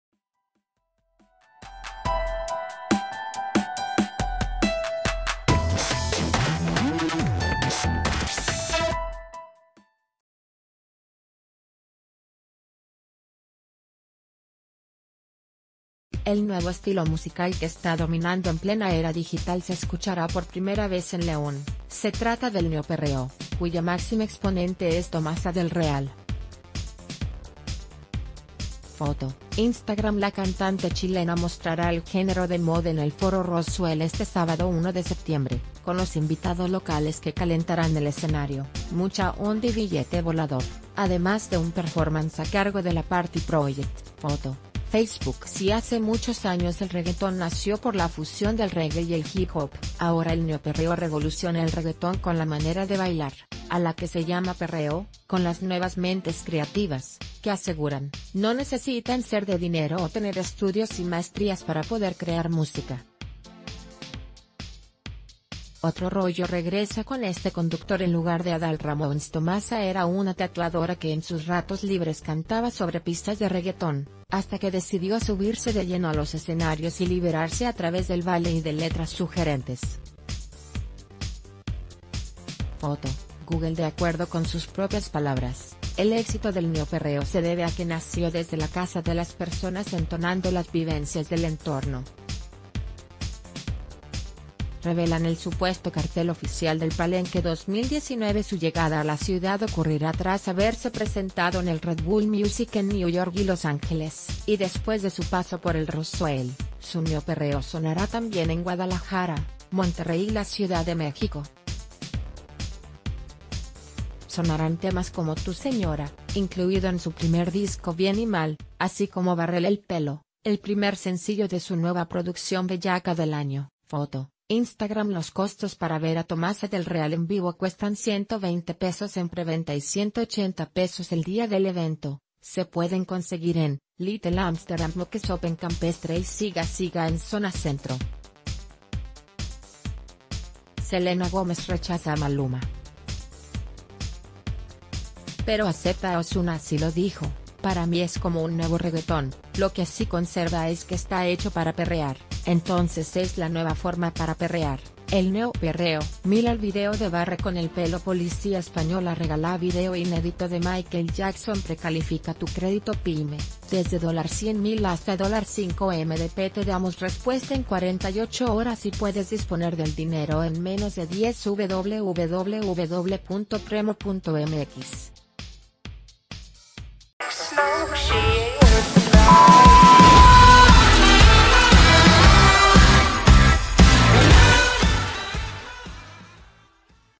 Tomasa del Real y su sexy 'neoperreo' pondrán a bailar en León
El nuevo estilo musical que está dominando en plena era digital se escuchará por primera vez en León, se trata del 'neoperreo', cuya máxima exponente es Tomasa del Real. Foto: Instagram La cantante chilena mostrará el género de moda en el Foro Roswell este sábado 1 de septiembre, con los invitados l...